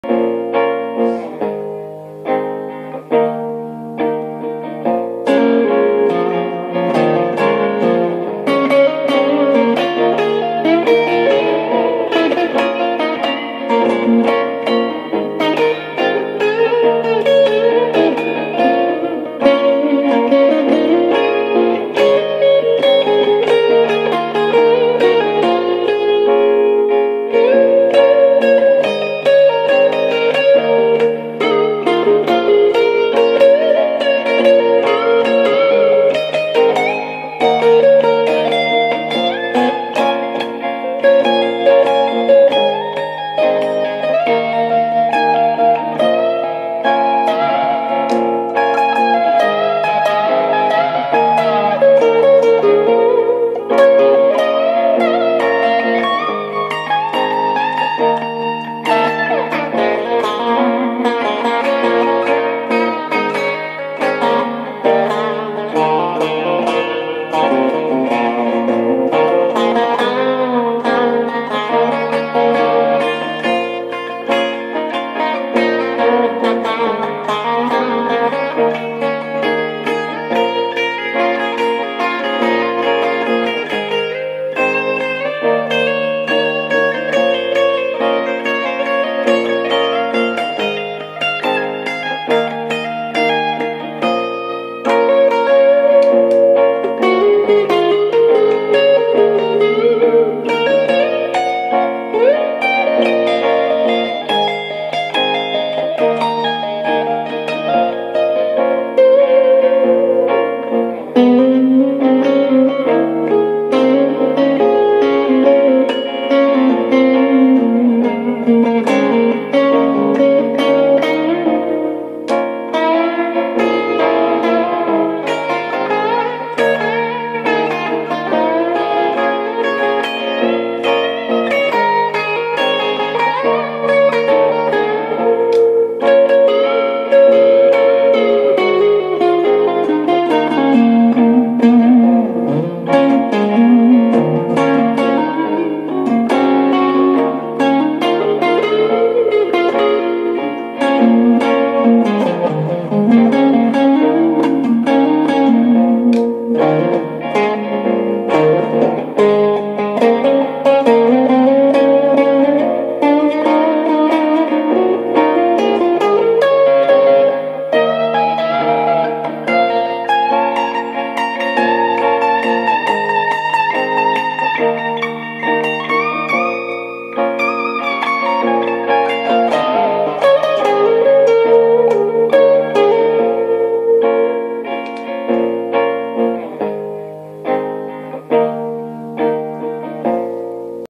Guitar improvisation with my new gear // Improviso com meus novos equipamentos - 2016
-Vintage AFD Paradise with Seymour Duncan Slash Signature pickups APH-II
-Bad Monkey (set to very low gain)
-Zoom G1Xon (only reverb and looper)
-Blackheart Handsome Devil Combo with Celestion G12 EVH speaker (clean and low volume)